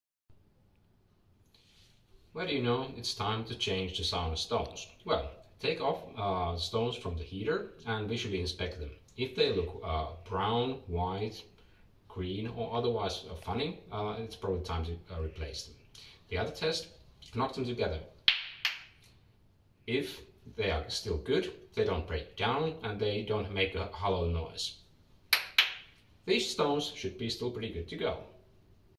How do you know it's the time to replace the sauna stones?
By Saunologia.fi, 2021